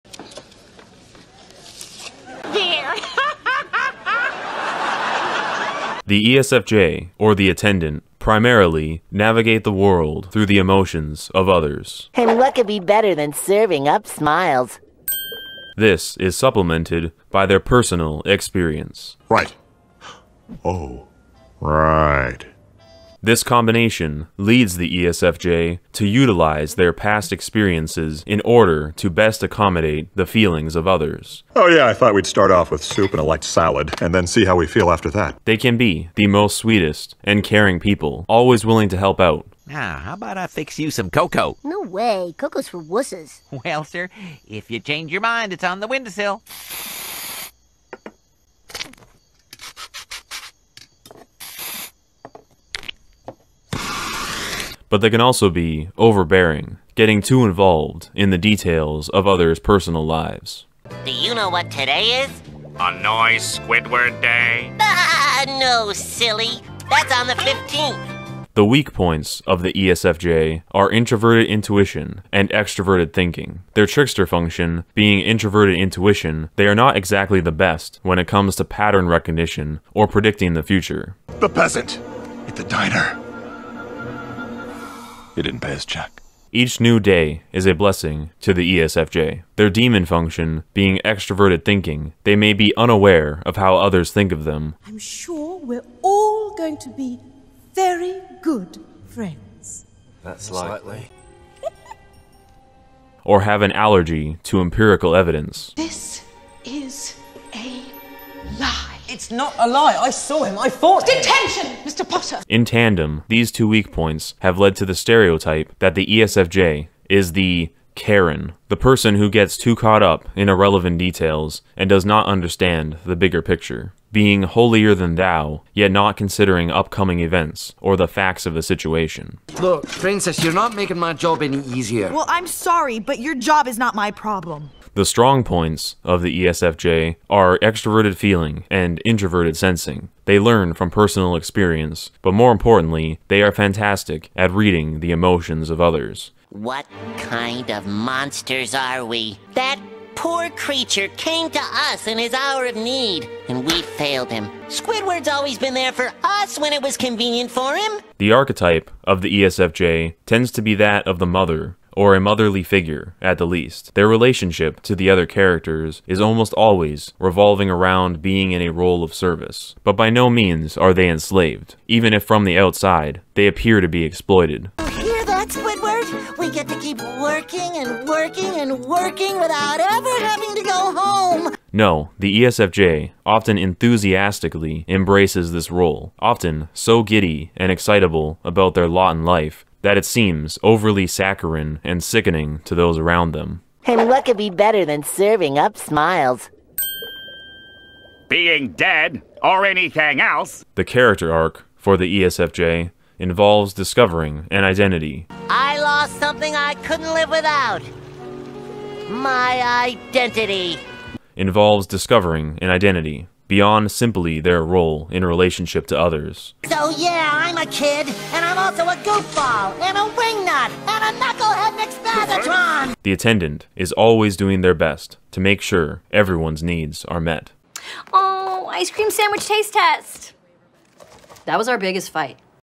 ESFJ Personality: The Attendant
A look at the ESFJ personality, what it means beyond a dry clinical analysis, and some examples and memes to truly flesh out the archetype. I do not currently make any money from this channel, so donations are appreciated.